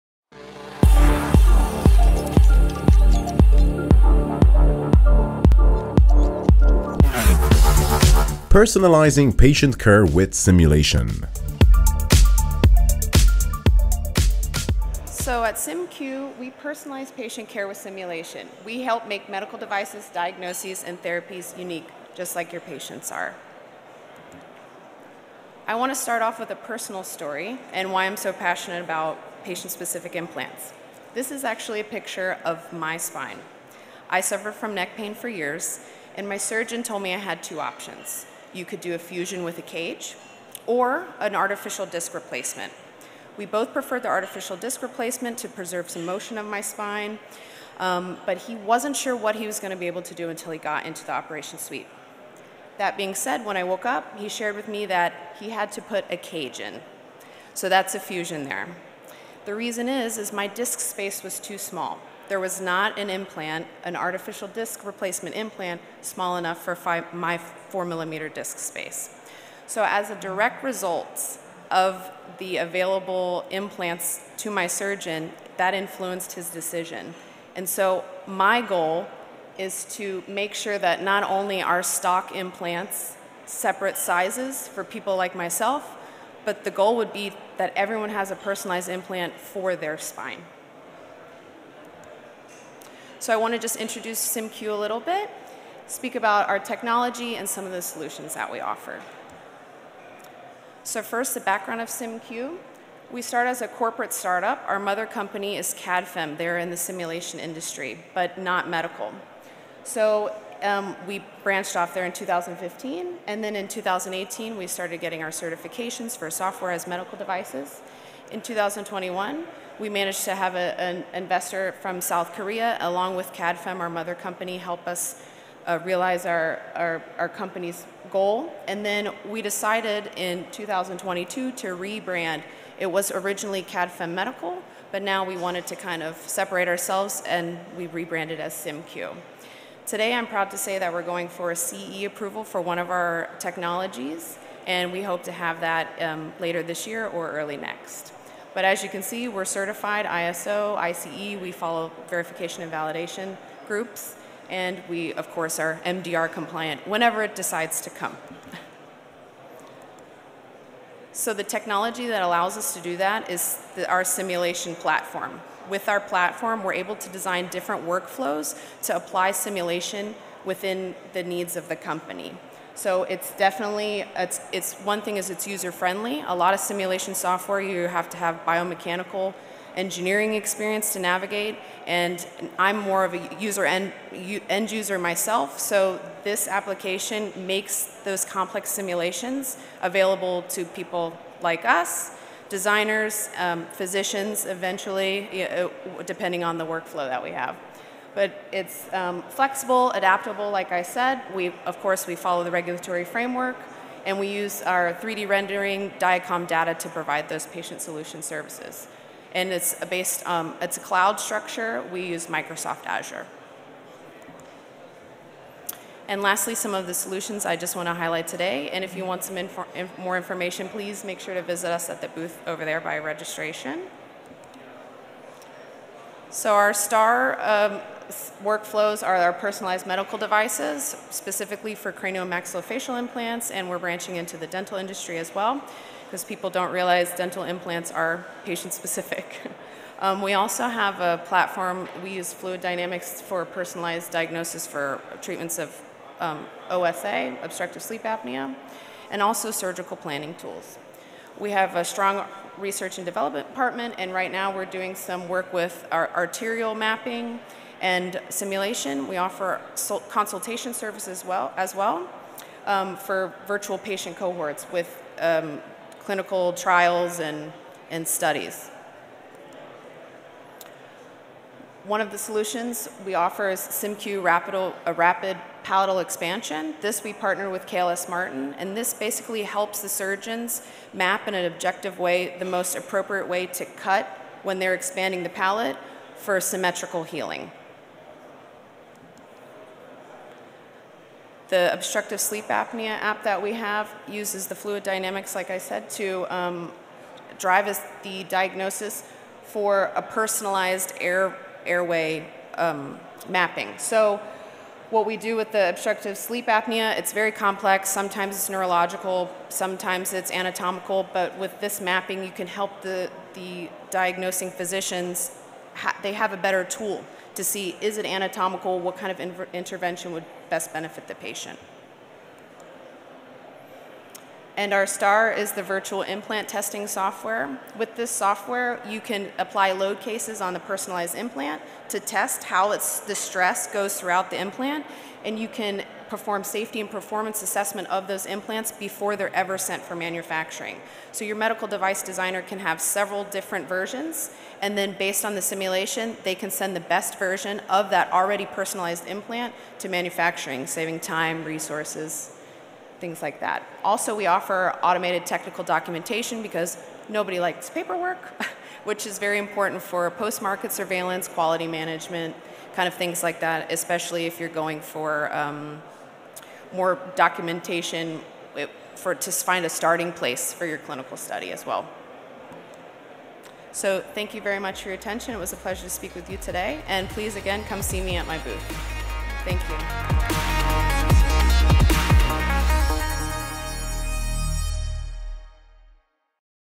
Personalized Patient Care: Keynote by Kelsey Crossman, SimQ Manager | MedTech Malta 2023
Welcome to Med-Tech World, where innovation transforms patient care! Join us for an insightful keynote presentation by Kelsey Crossman, Business Development Manager at SimQ, as she explores the power of simulation in personalizing patient care. In this enlightening talk, Crossman delves into how simulation technology is revolutionizing healthcare delivery, enabling tailored interventions and enhancing patient outcomes. Join us as we unlock the potential of simulation in shaping the future of patient-centered care with Kelsey Crossman. MedTechWorld SimulationTechnology PatientCare KeynoteSpeaker"

➡️ Next stop:
Dubai, UAE | 26 Feb 2024 | Roadshow
Valletta, Malta | 6-8 Nov 2024